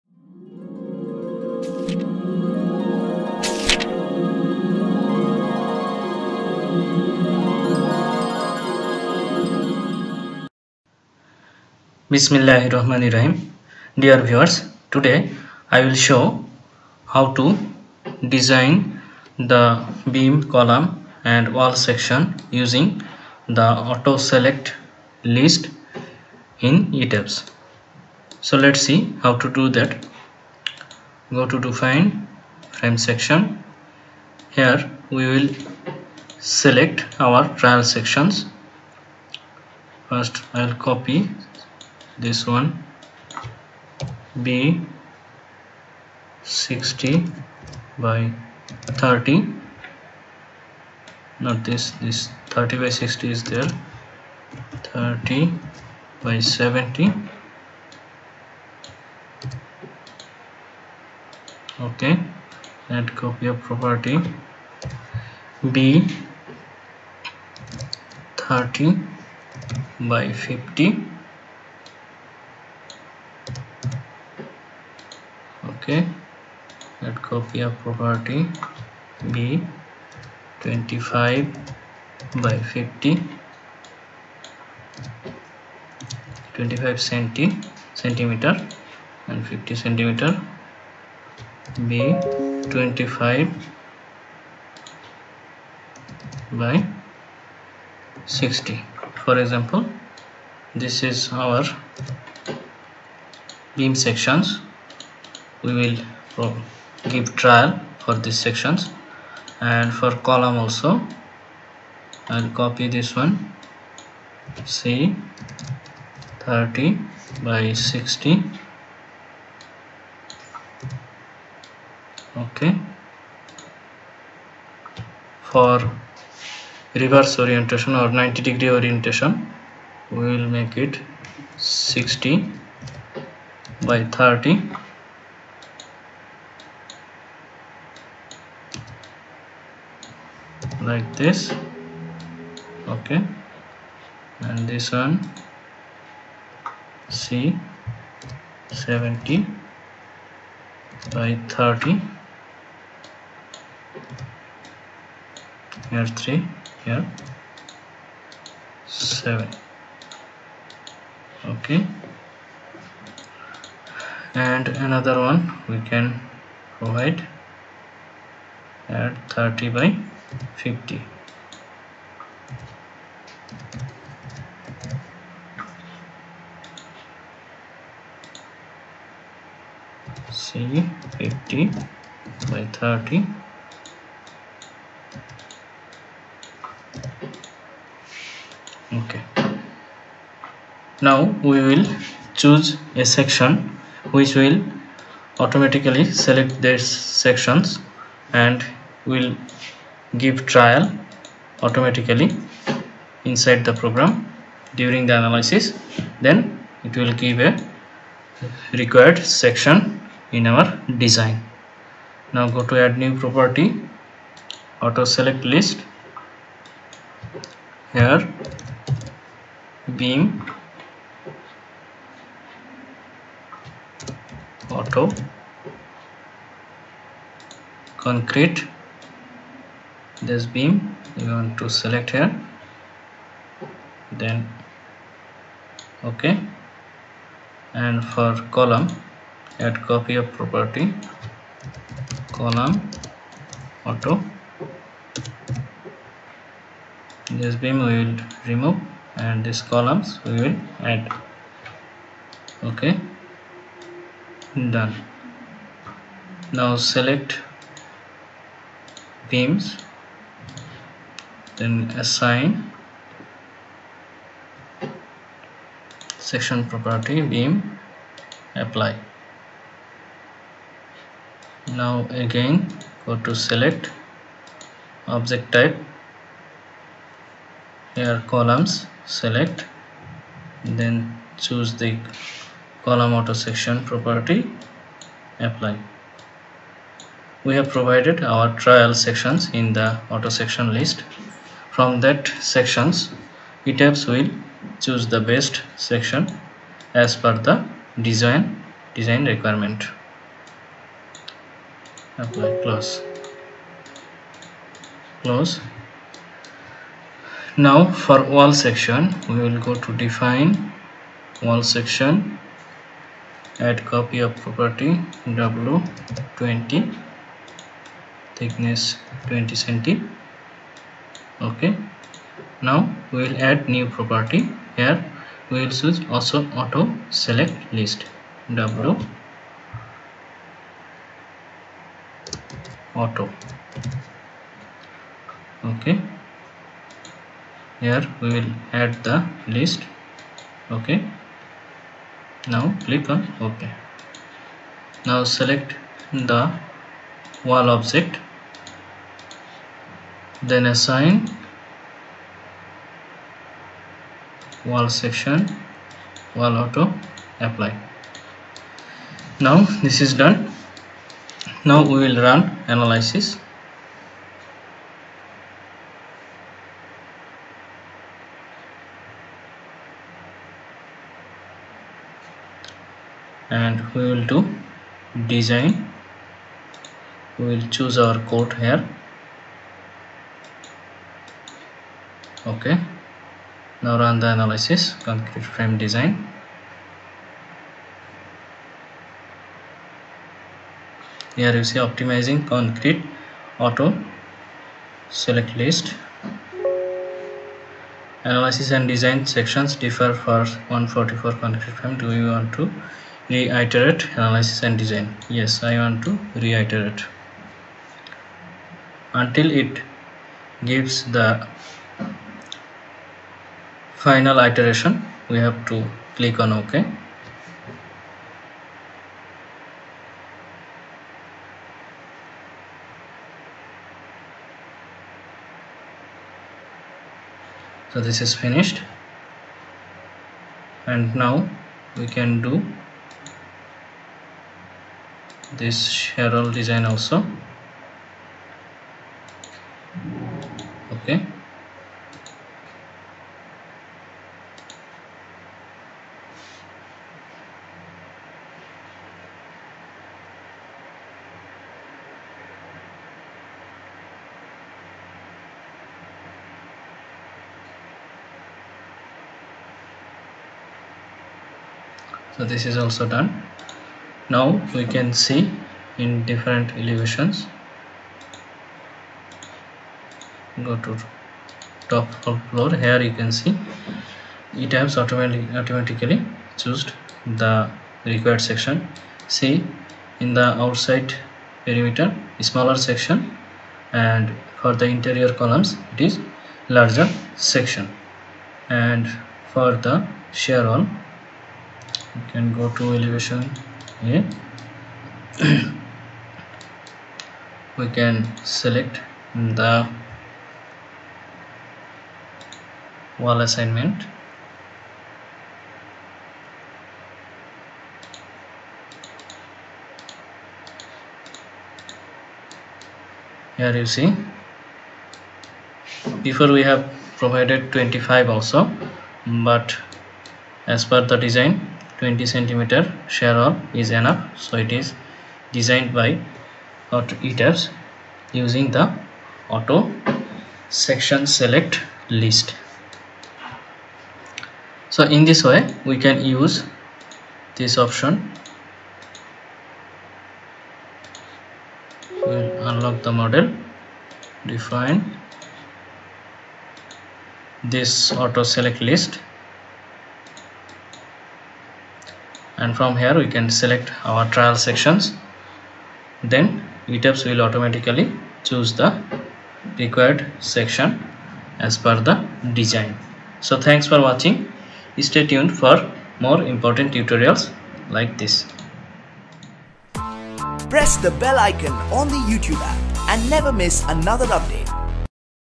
DESIGN FRAME AND WALL SECTIONS USING AUTO SELECT LIST IN ETABS
WATCH TILL THE END TO LEARN HOW TO DESIGN MEMBERS IN ETABS WITHOUT CHANGING SECTIONS EVERY TIME.